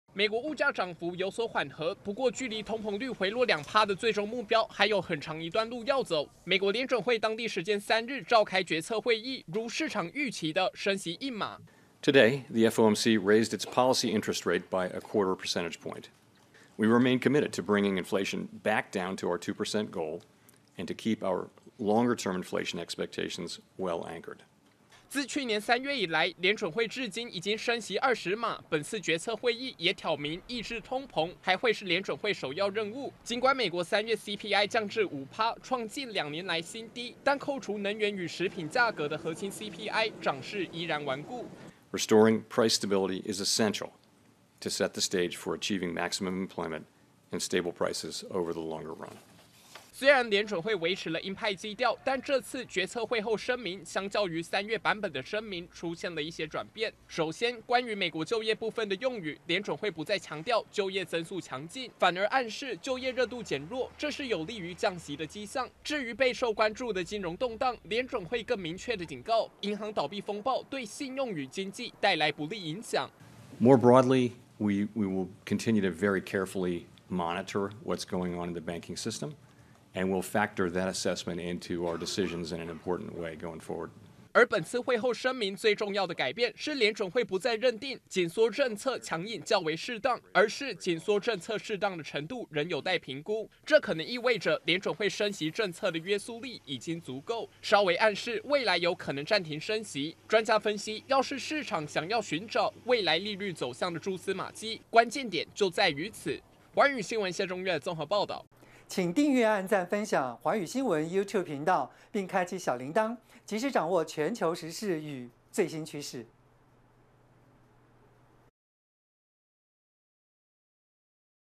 Fed如預期再升息1碼 略暗示可能暫停升息｜寰宇新聞
美國聯準會3日召開決策會議，宣布升息1碼，將基準利率調到5到5.25%區間。聯準會主席鮑爾強調，目前優先任務，依然是對抗通膨，因為美國「核心CPI」漲勢頑強，顯現物價漲幅太高。雖然聯準會繼續保持「鷹派 」態度，但這一次的會後聲明，卻稍微暗示了，未來暫停升息的可能性，意味著1年多來的緊縮政策，已經達成一定的約束力。